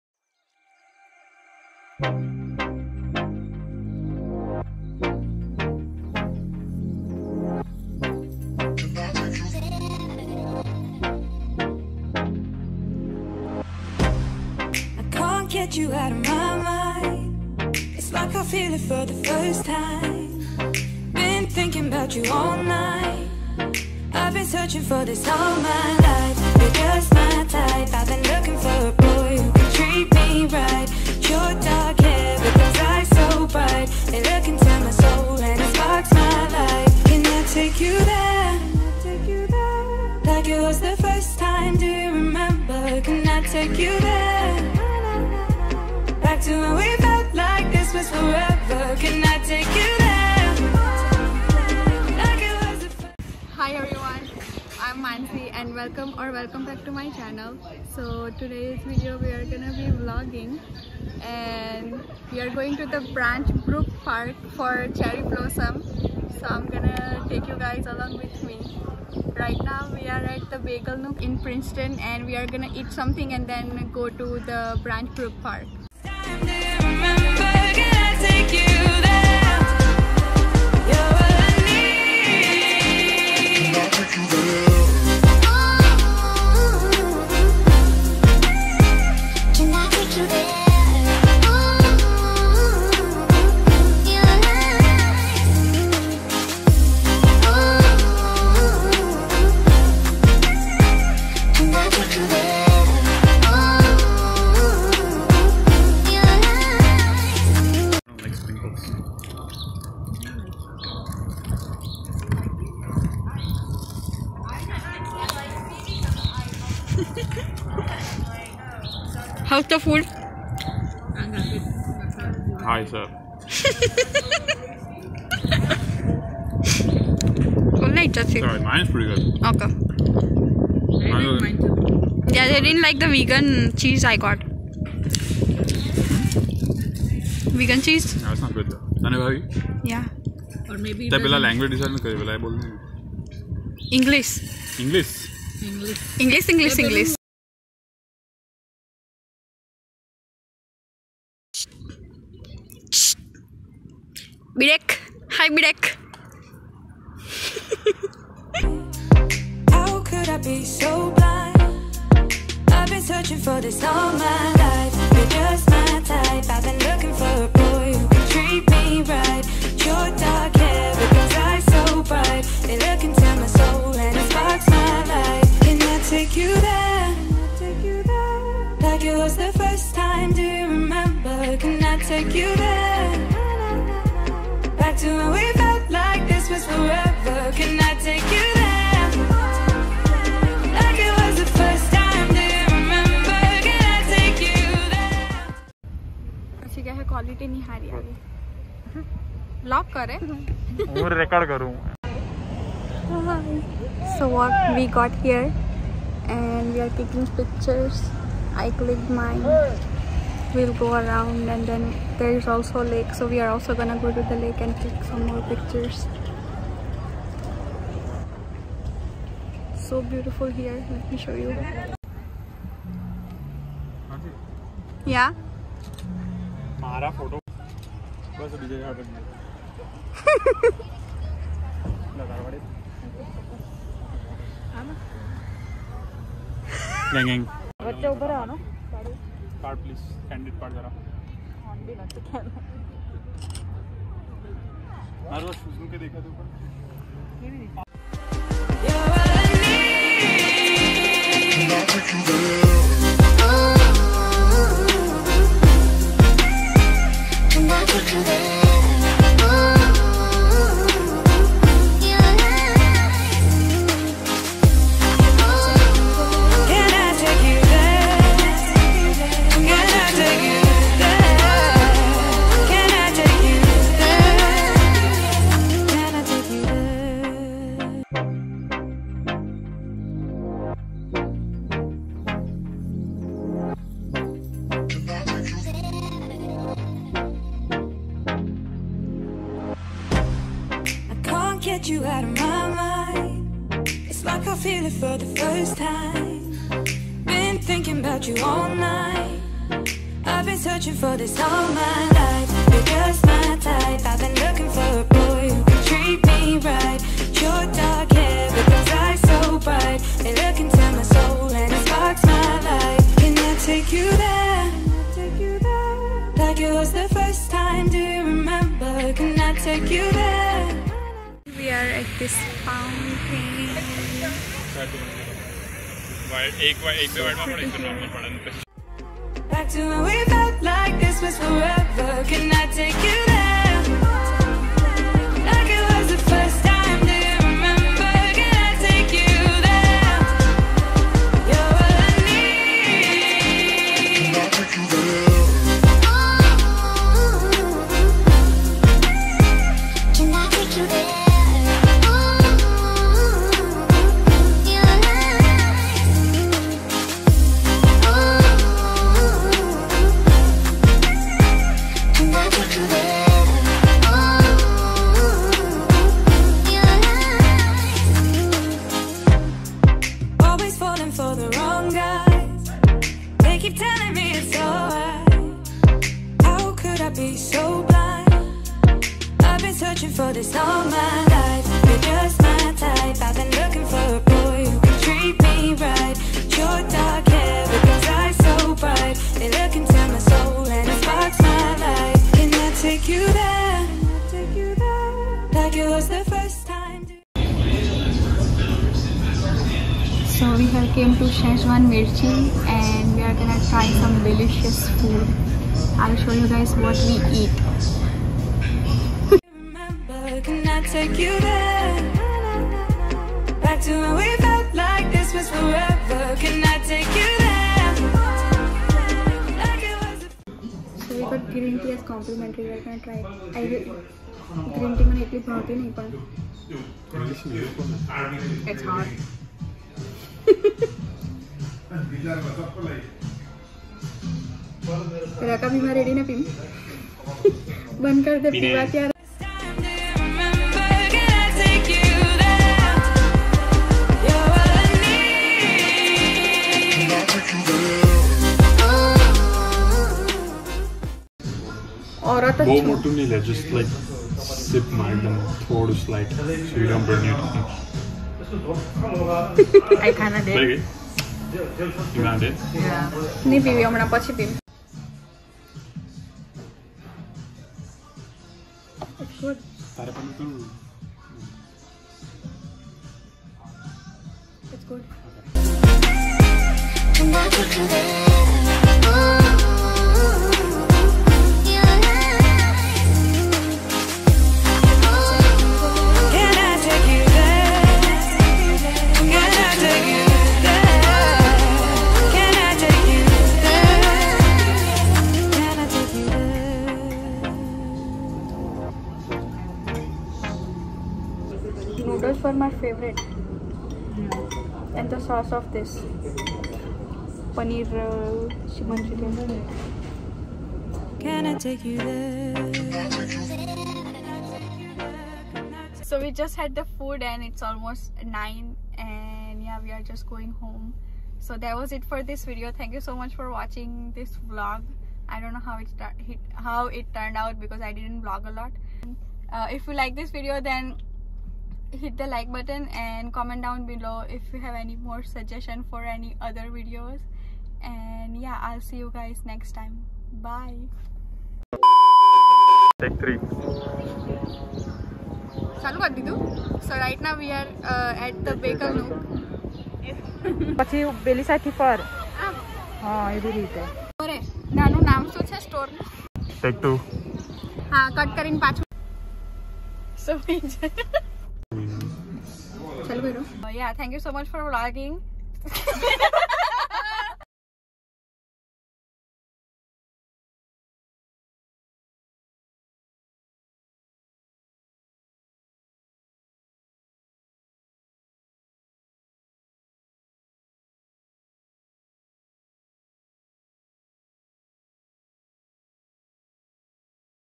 Branch Brook Park Cherry Blossom in New Jersey 2021 | Dinner and much more | Mansi Patel
This video is about the one of the most famous cherry blossom parks in new Jersey, The Branch Brook Park. Every year in spring, this place is bloomed with thousands of cherry blossom trees across the whole park. I had privilege to visit this park during the Spring season and explore Jersey City around the park.